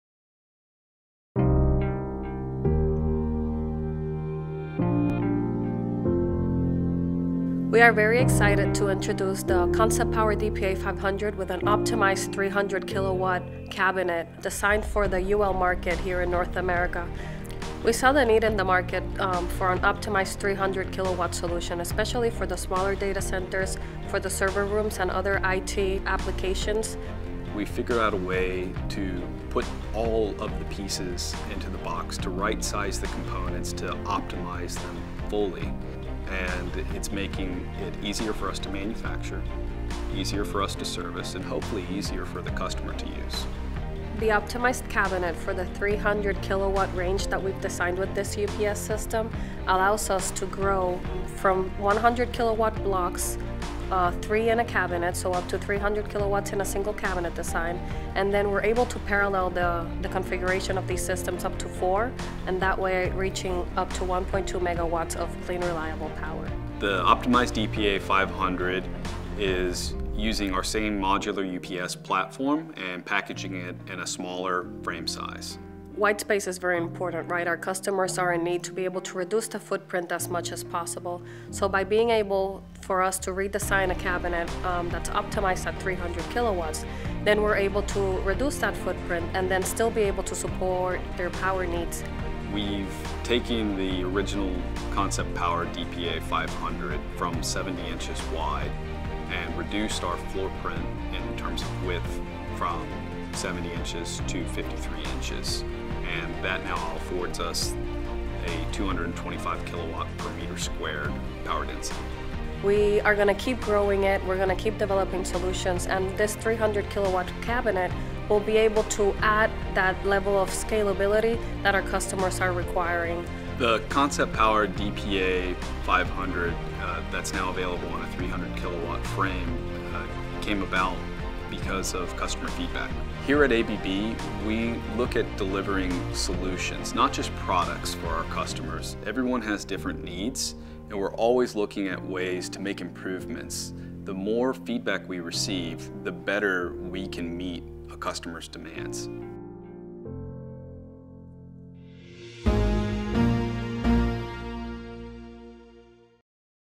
ABB expands the 480V Conceptpower DPA 500 with an optimized 300kW solution
ABB expands its UPS offering to include an optimized modular 300kW solution for mid-sized data centers, server rooms and other IT infrastructure applications. Building on the true online, double conversion UPS design of the Conceptpower DPA 500 480V UL UPS, this compact 300kW system allows for three 100kW module sets within each UPS cabinet; up to four cabinets can be configured in parallel for up to 1.2MW of clean reliable power.